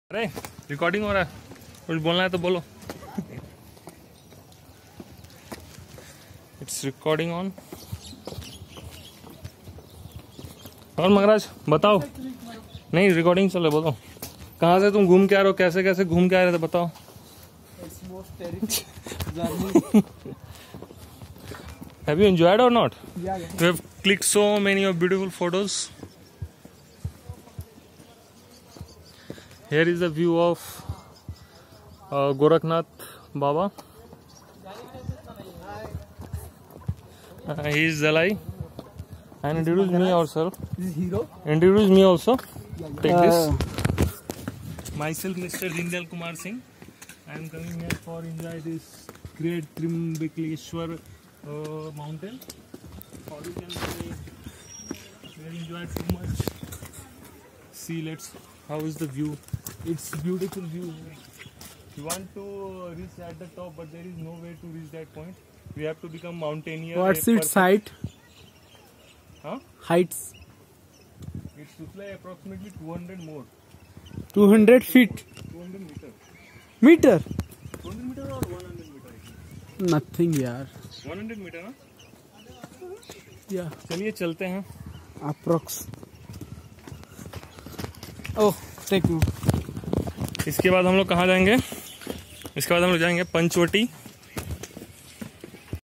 Nashik Trimbekeshwar | Brahmgiri Mountain tracking / Godawari River origin point Maharashtra tourism
Near Trimbekeshwar Jyotirling
Maharashtra tourism
Nashik, Maharashtra
Panchvati, Nashik, Maharashra